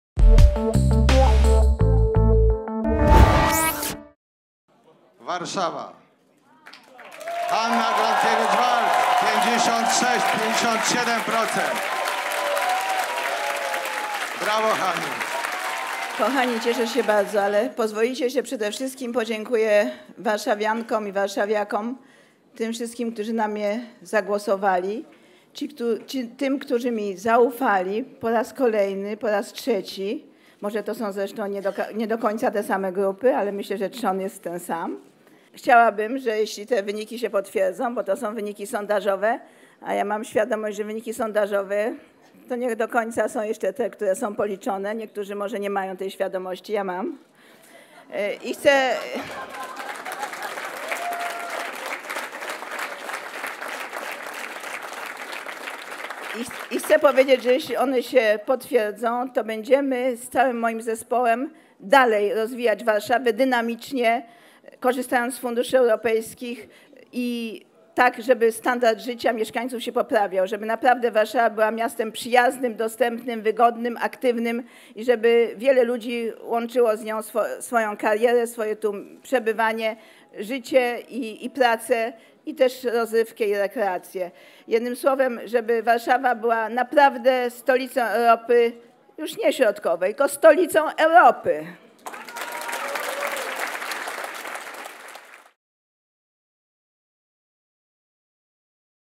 Wybory 2014. Hanna Gronkiewicz-Waltz wygrywa w Warszawie. Szał radości w sztabie
Hanna Gronkiewicz-Waltz po raz trzeci prezydentem Warszawy. W drugiej turze wyborów uzyskała 342 857 głosów - podała oficjalnie Miejska Komisja Wyborcza.